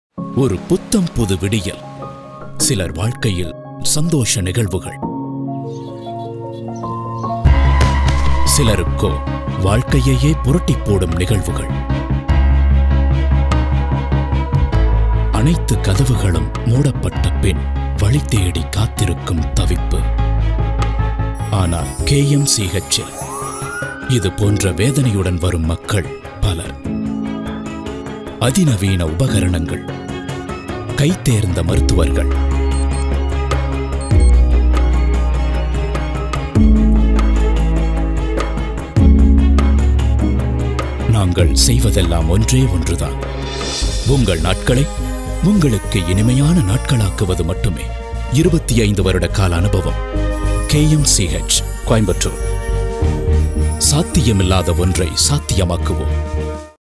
Ad Film for KMCH (Kovai Medical Center & Hospital), Coimbatore
G3 Media Works is a video content creation production house. This is one of our work for a healthcare brand which is very popular. KMCH (Kovai Medical Center & Hospital)  is the most trusted Multispecialty Hospital in the Southern Indian City of Coimbatore.